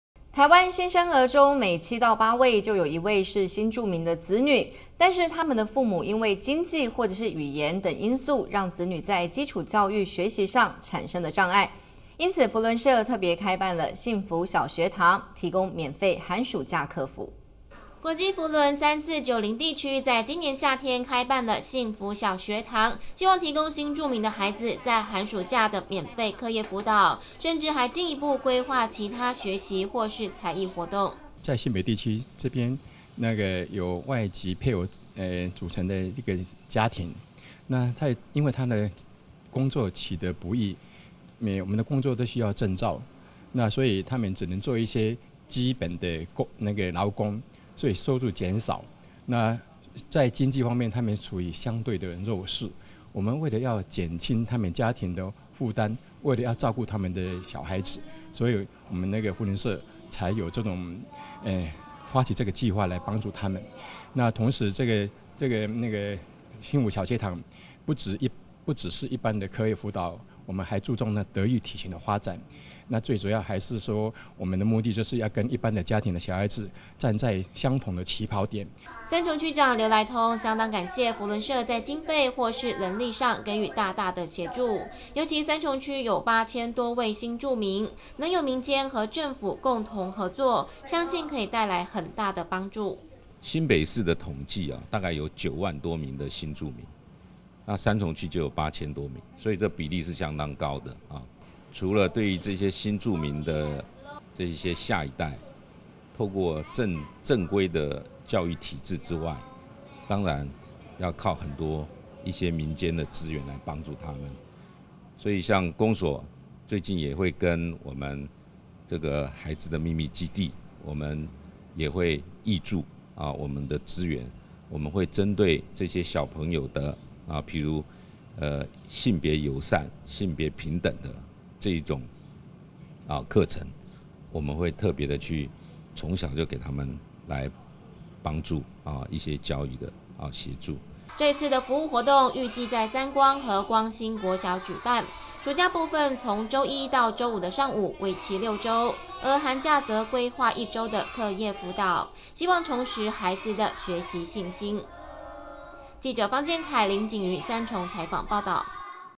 天外天新聞1060712 05幸福小學堂 協助弱勢學童
台灣新生兒中每七到八位就有一位是新住民子女，但他的父母因為經濟或是語言等因素，讓子女在基礎教育學習上產生障礙，因此扶輪社特別開辦「幸福小學堂」提供免費寒暑假課輔。